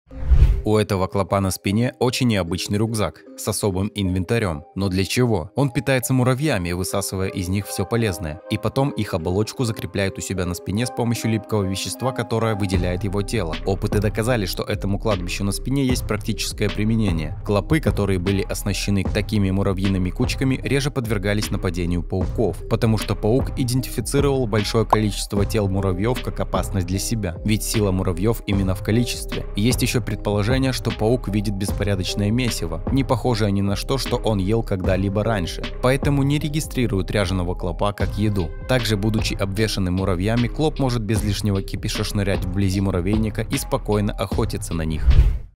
Зачем клоп носит на спине гору муравьев
Acanthaspis petax